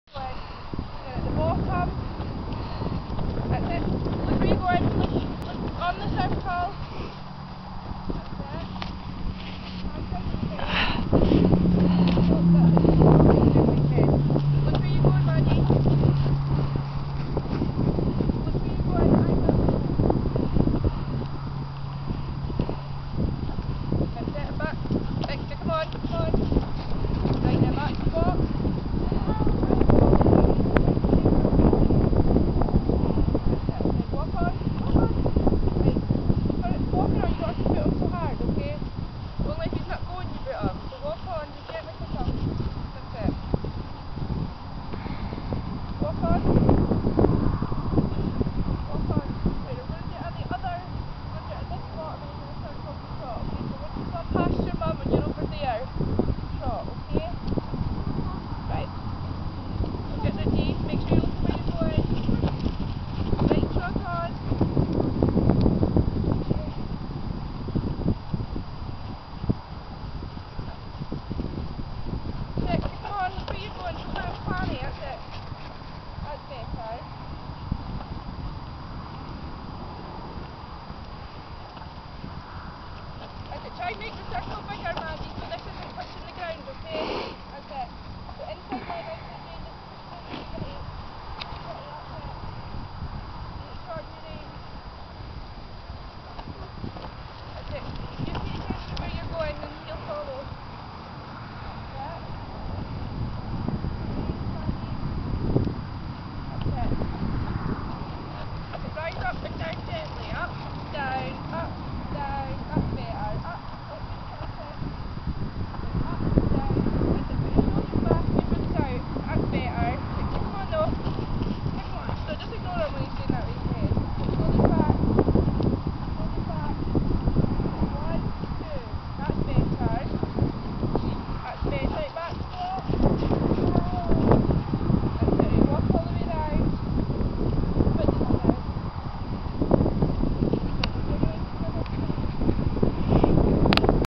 Maddy's first ride on Smokey!
This is my daughter Maddy  on her new pony Smokey. This is the first time she has ridden him since getting him  home a week ago, and he's not been ridden for 9 months so I think they did well together  for a first attempt. The camera died on me, but after being on the lunge, she rode off it and it was ok.
He's a little sod, but with some work and patience, will turn out fine for her I think.